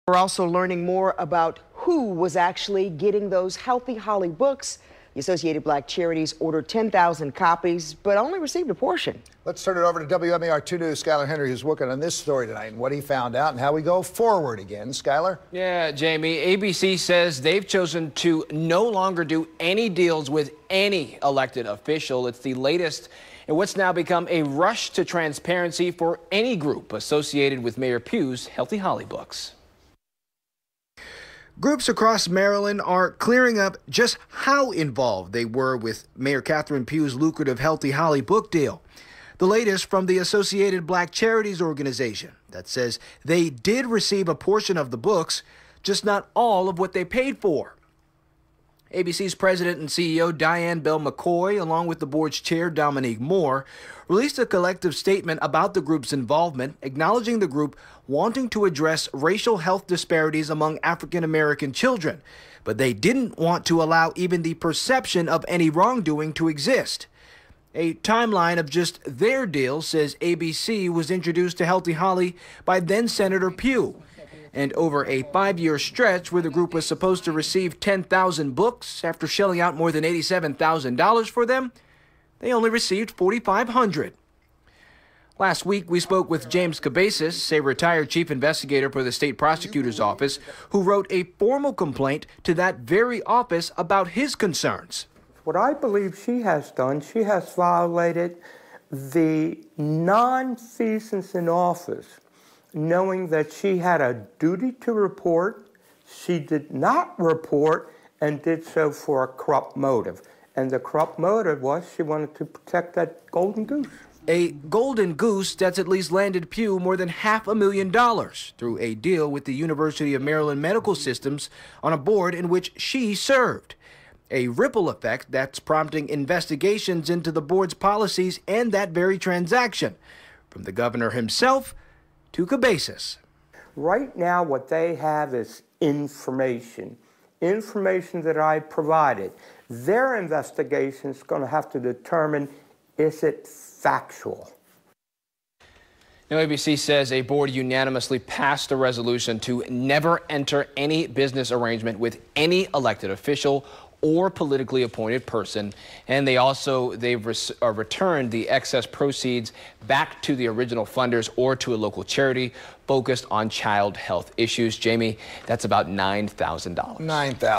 Associated Black Charities received 4,500 of the 10,000 'Healthy Holly' books it purchased
Associated Black Charities plans to return the money that remains from donations given to them to pay for "Healthy Holly" books.